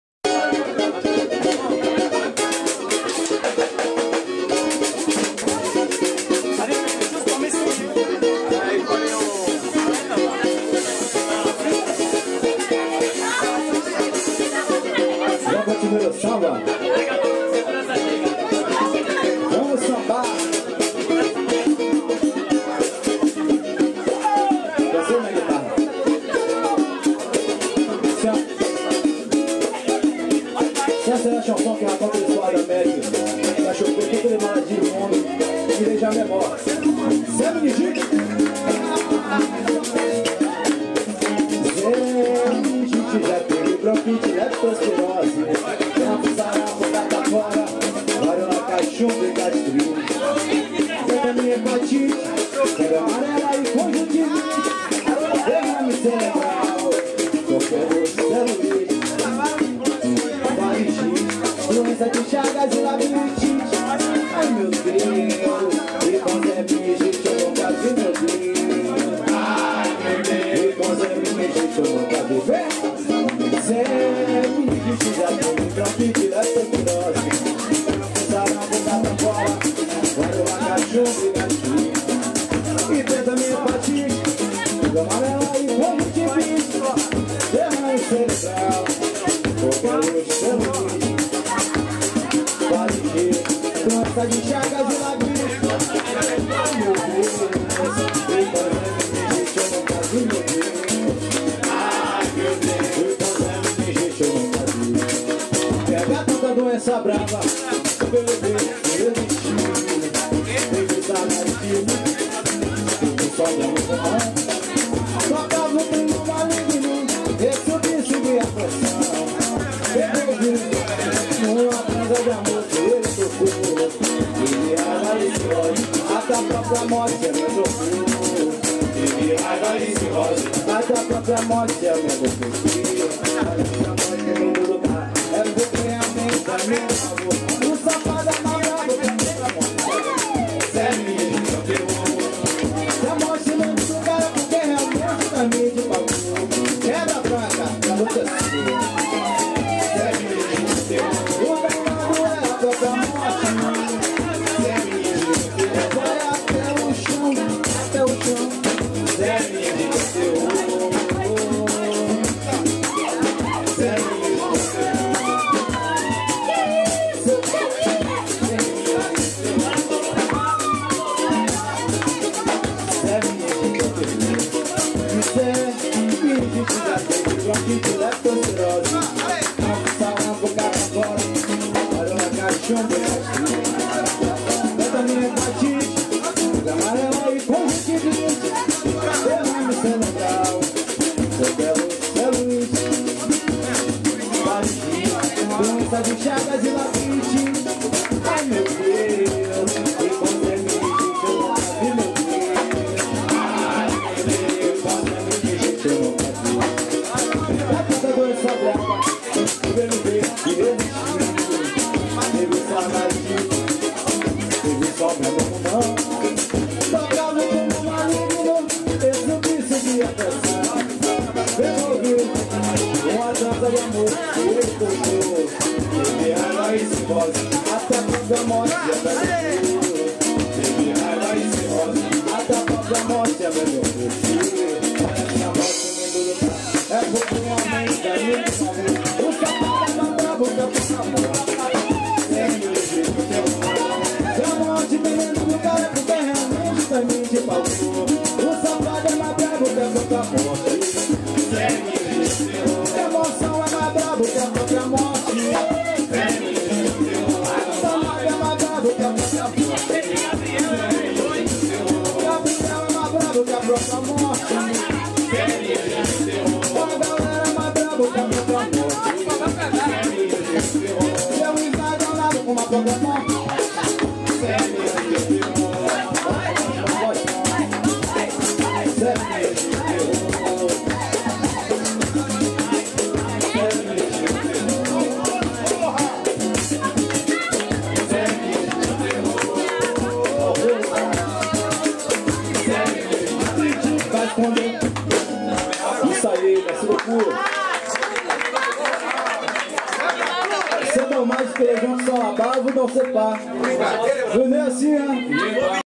BRAZIL TIME SAMBA by Pagode do Jambo CASA LATINA Bordeaux
2 concerts pour une soirée brésilienne !

BRAZIL TIME à la CASA LATINA ( bordeaux)

21H00 BAL BRESILIEN !! avec le groupe PAGODE DO JAMBO

MINUIT : TAINOS TIME (Les TAINOS font le show toute la fin de soirée)

CASA LATINA devient pour la soirée CASA DO BRAZIL !
avec les musiciens du groupe PAGODE DO JAMBO !
La voix et la danse sont à l'honneur comme dans la plupart des musiques brésiliennes. !

PAGODE DO JAMBO, c'est 5,6 musiciens passionnés par leur pays et leurs traditions !!
TAÏNOS LE GROUPE LATINO DE BORDEAUX qui déchire avec une ambiance torride!!